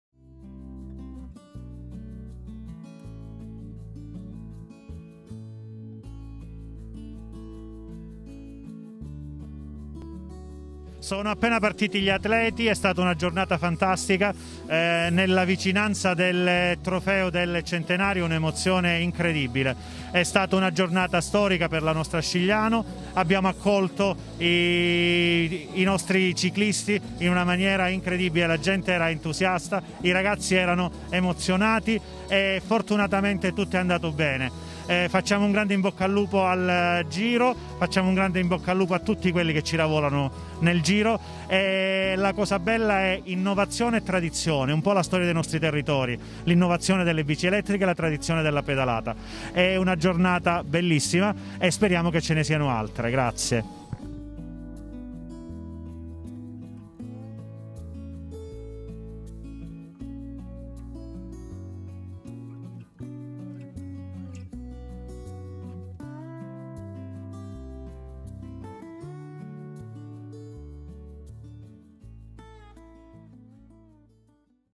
Raffaele Pane, vicino al trofeo - Giro E 2020 - 4° Tappa: Scigliano - Camigliatello
A cura di Antonio Abbruzzese - 7 ottobre 2020